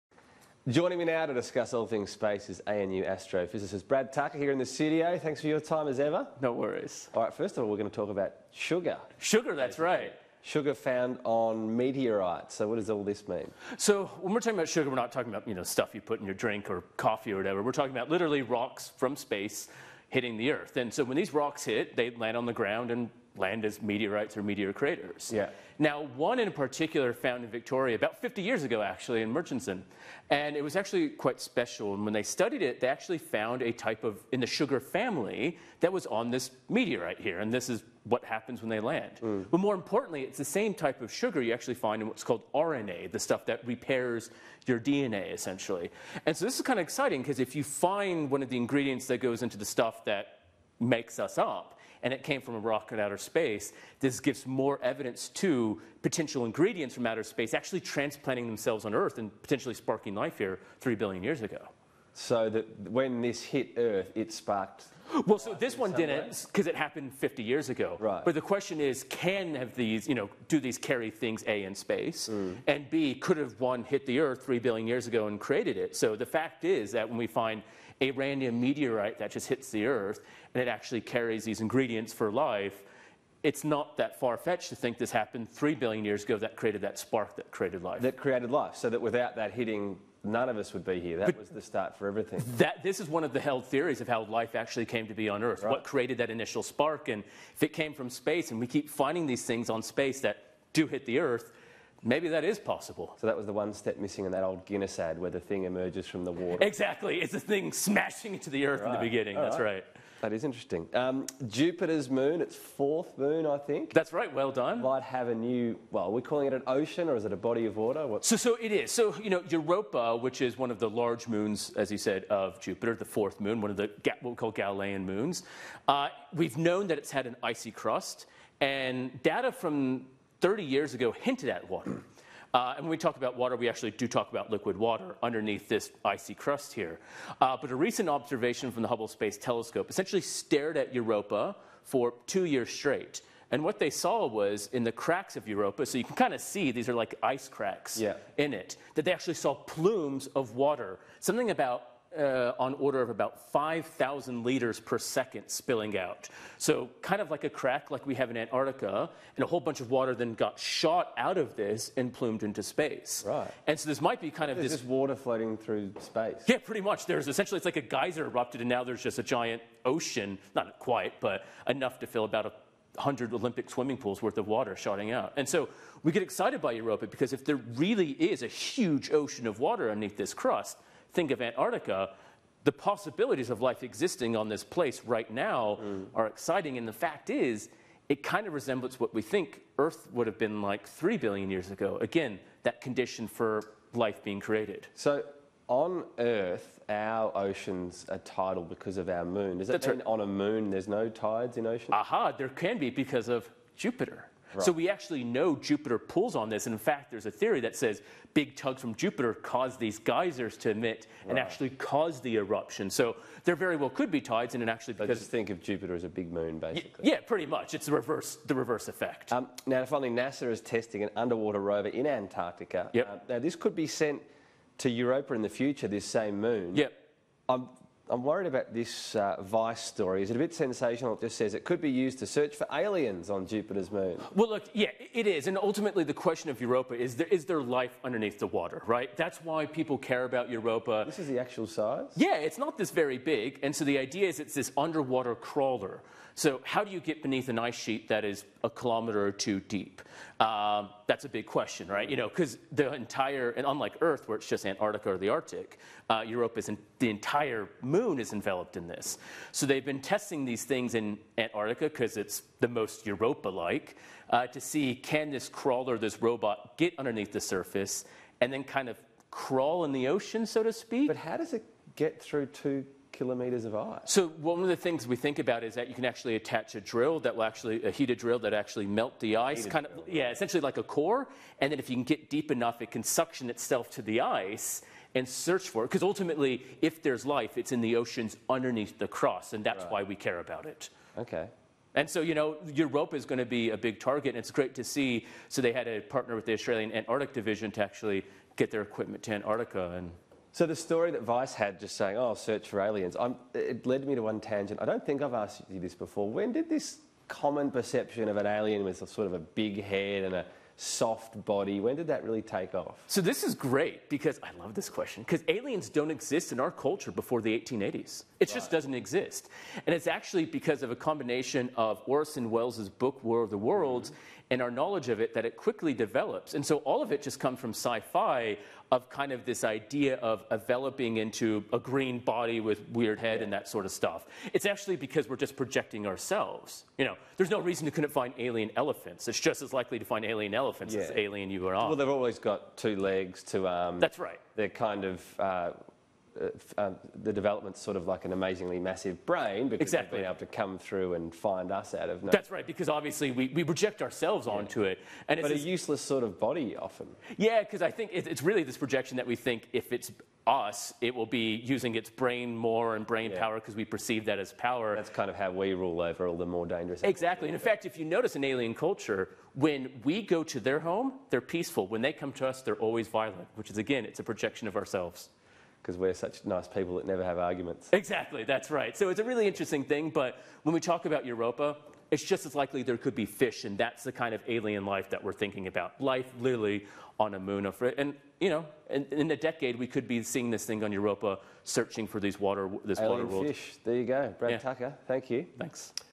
Space Watch: Sugar meteors, water on Jupiter's moons and an alien-hunting robot
Data from Jupiter's fourth moon, Europa, has located a geyser-like crack on its surface that appears to have ejected a plume of water into space.